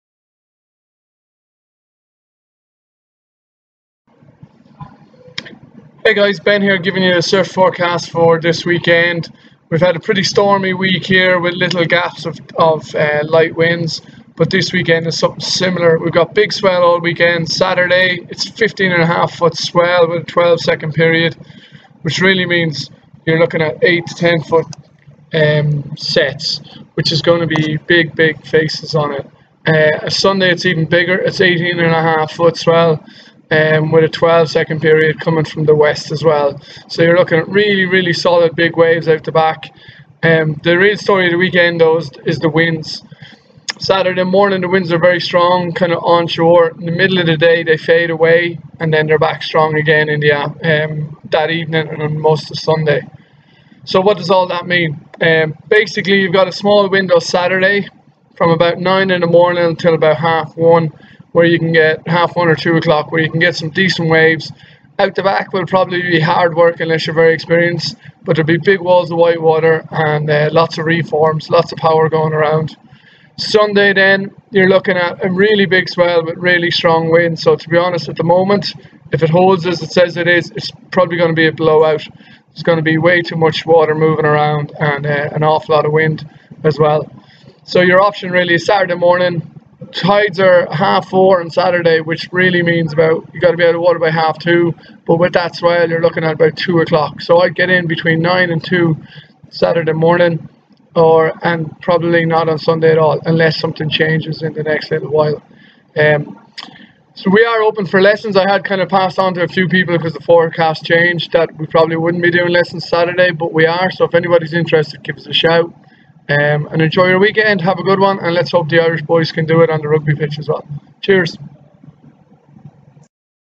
lahinch surf forecast feb 25 26th 2017
weekly surfing forecast for lahinch co clare , on the wildatlanticway